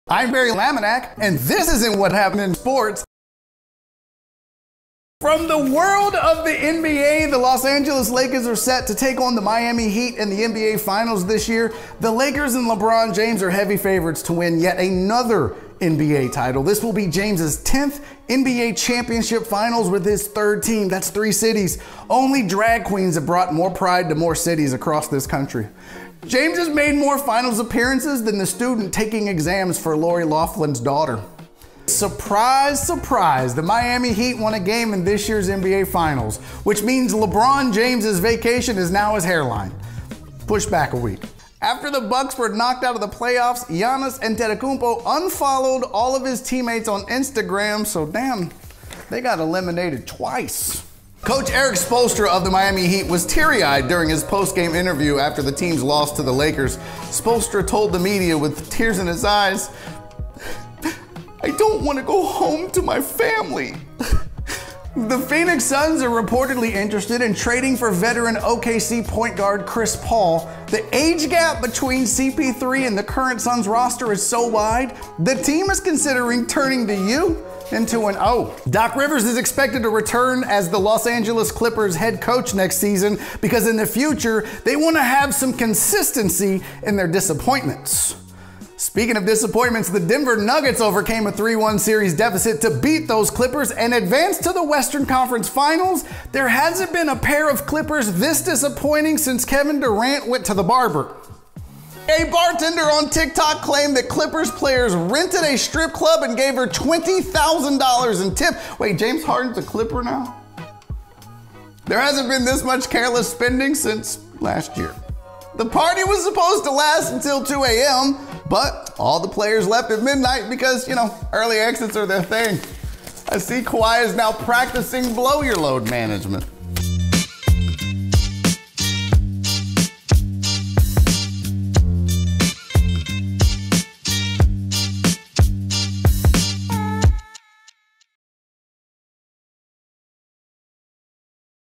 The Best of Barry On Sports 2020 (6 of 10) - NBA Jokes Edition
Here is part 6 (of 10) in a series that features the BEST jokes from Barry On Sports for 2020.

These are the best NBA jokes from 2020. We got after Lebron James (and his hair), The Los Angeles Lakers, Lori Loughlin, Miami Heat, Giannis Antetokounmpo (The Greek Freak), Chris Paul, The Phoenix Suns, Doc Rivers, Erik Spoelstra, The Los Angeles Clippers, and more!